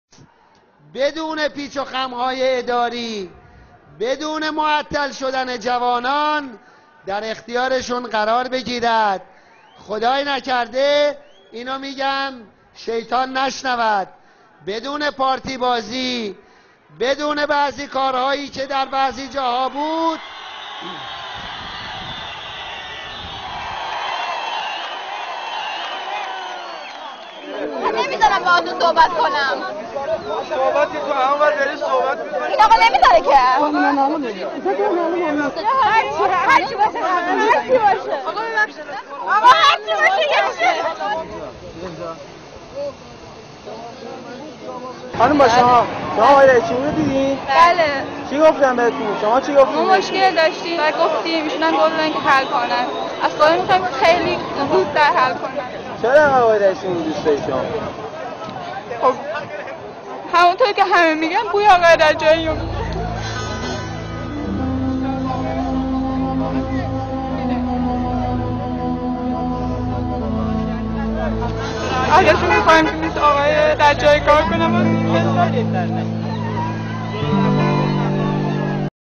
ardabil 2
Province visit is the efficient innovation of president ahmadinejad. It was once imitated by Sarkozy and Gordon Brown. This video shows Ardebil province people welcome the president.